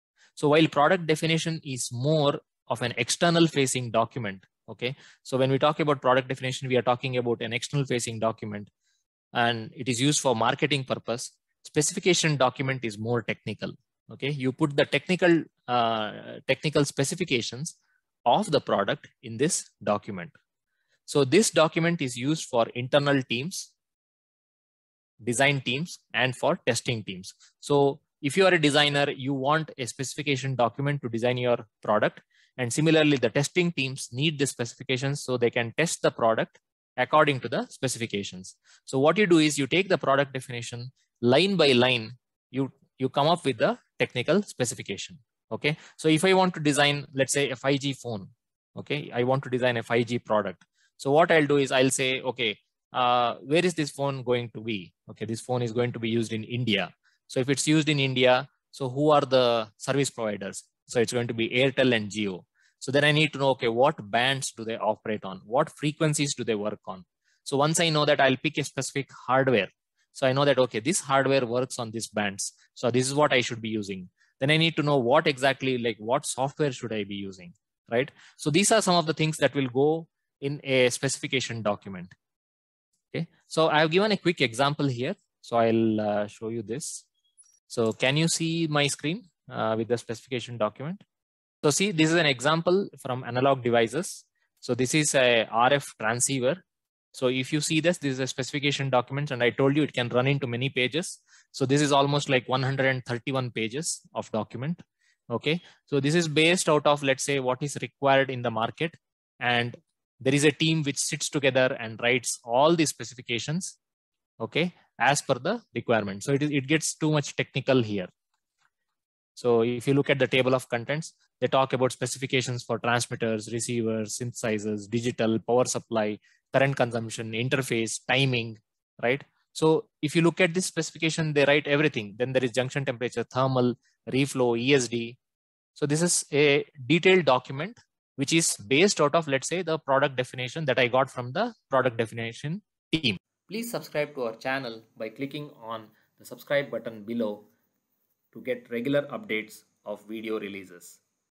4_Specification Definition
After deciding which electronic product need to be designed, the required specification or what is what should be defined first. What are the points that need to be considered during the process of defining the specifications is the topic of this video.